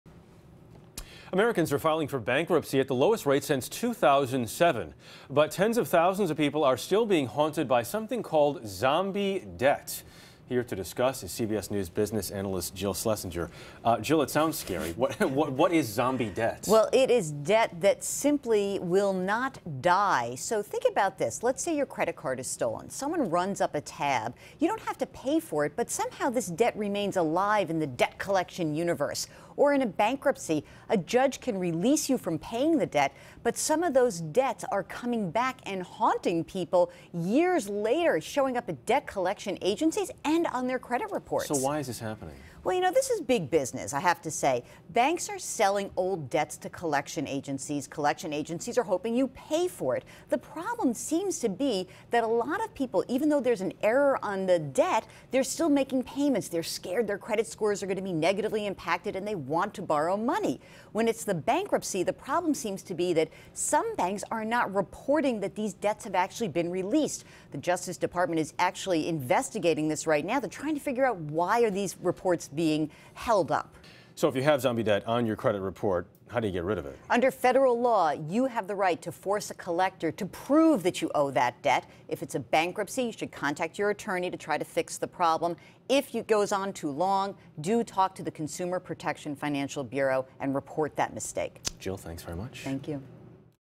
Are Americans haunted by “zombie debt”?
Americans are filing for bankruptcy at the lowest rate since 2007. But tens of thousands of people are still being haunted by something called “zombie debt.” CBS News business analyst Jill Schlesinger explains why that may be.